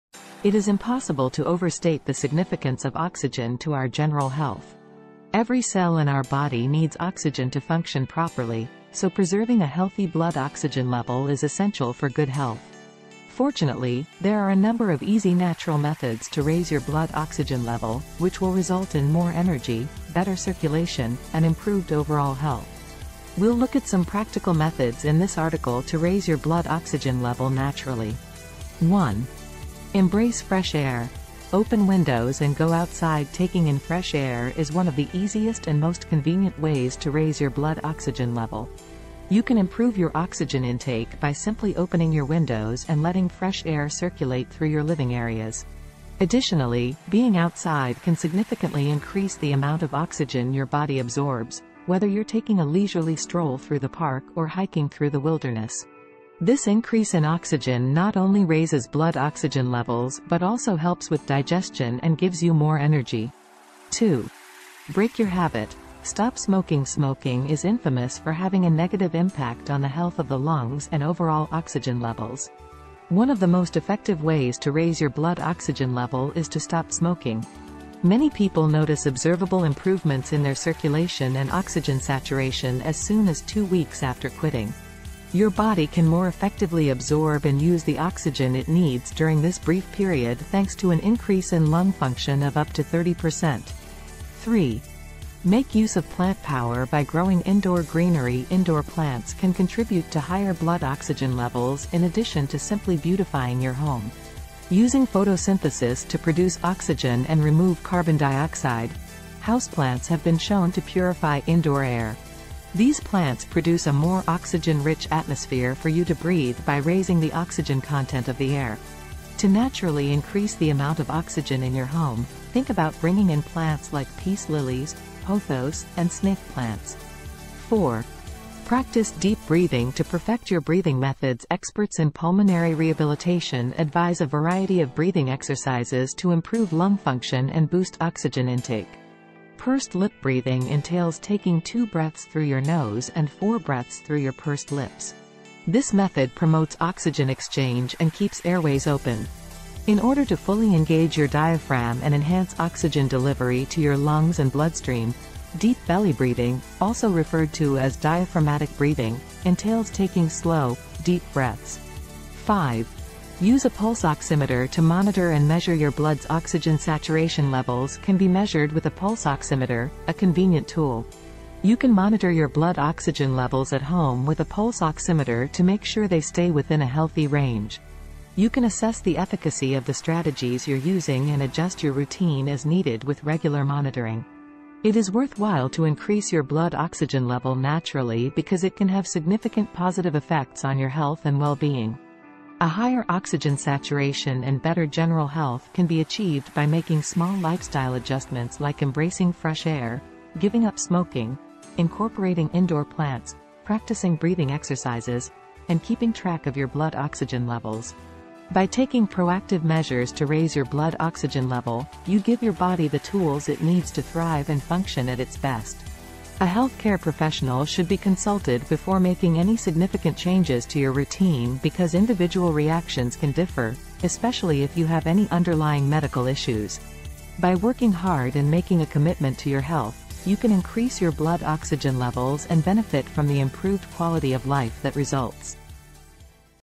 Tips to Increase Your Blood Oxygen Level Naturally
It is impossible to overstate the significance of oxygen to our general health. Every cell in our body needs oxygen to function properly, so preserving a healthy blood oxygen level is essential for good health. Fortunately, there are a number of easy natural methods to raise your blood oxygen level, which will result in more energy, better circulation, and improved overall health. We'll look at some practical methods in this article to raise your blood oxygen level naturally.